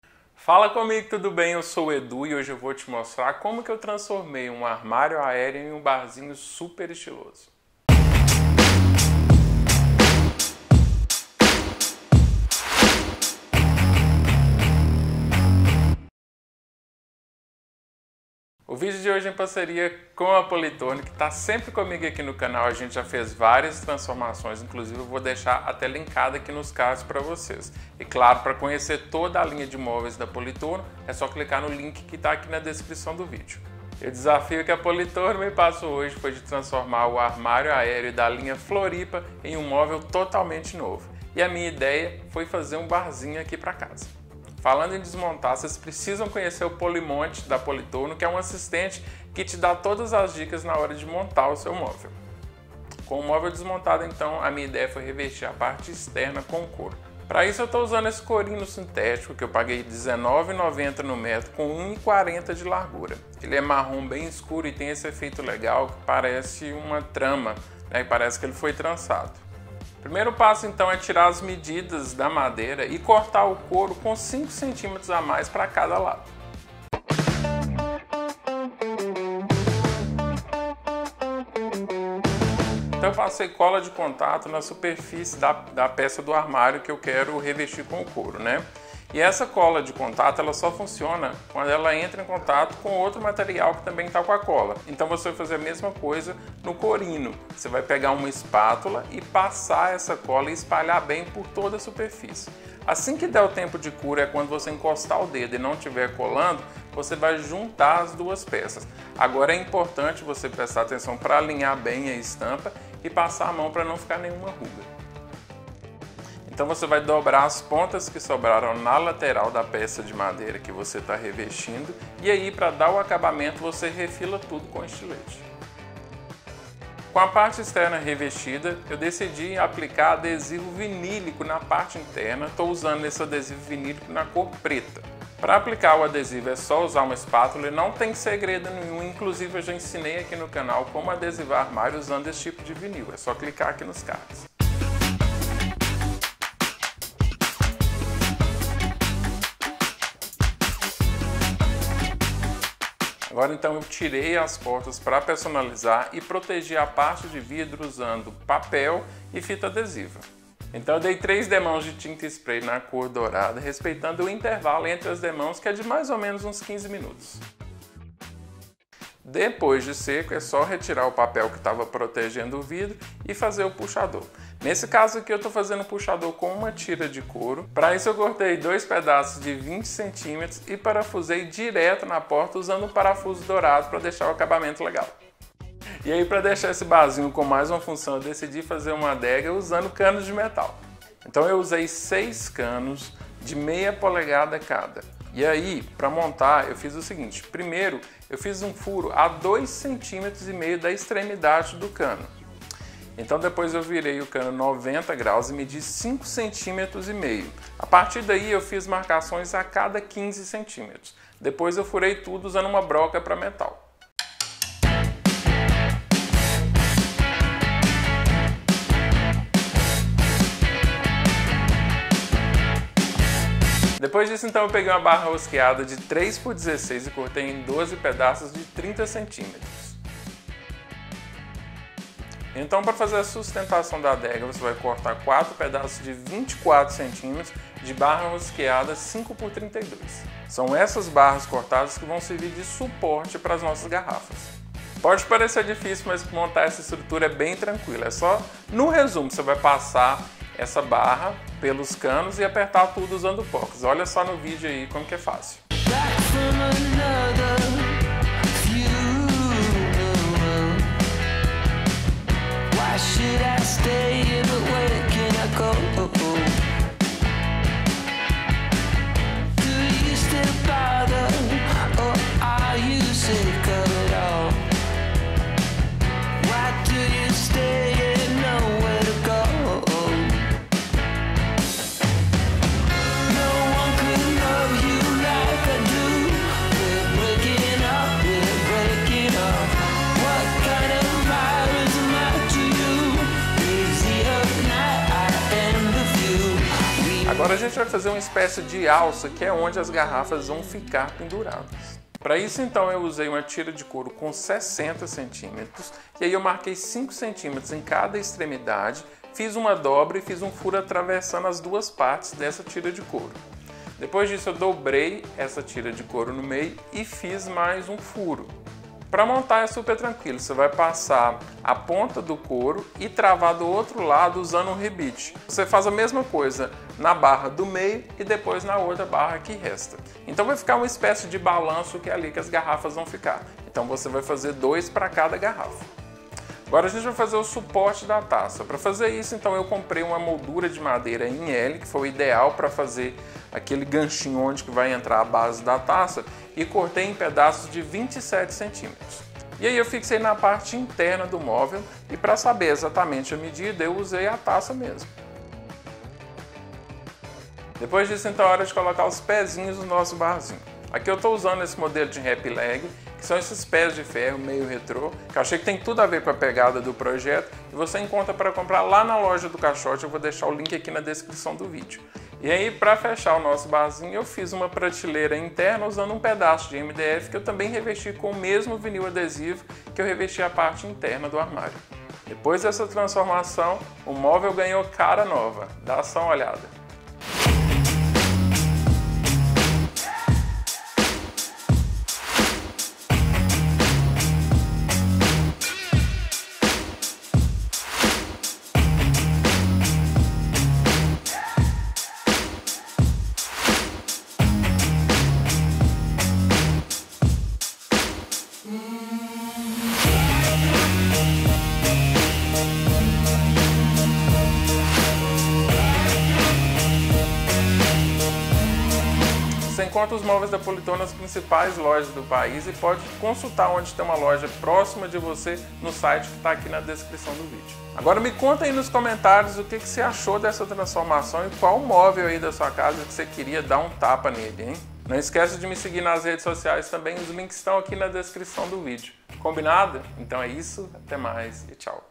COMO TRANSFORMEI UM ARMÁRIO AÉREO EM UM BARZINHO INCRÍVEL | DIY
Hoje eu mostro pra você a resignificação que fiz com um móvel: transformei um armário aéreo em um Barzinho.

Caixa Postal 1978, CEP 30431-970 , Belo Horizonte - MG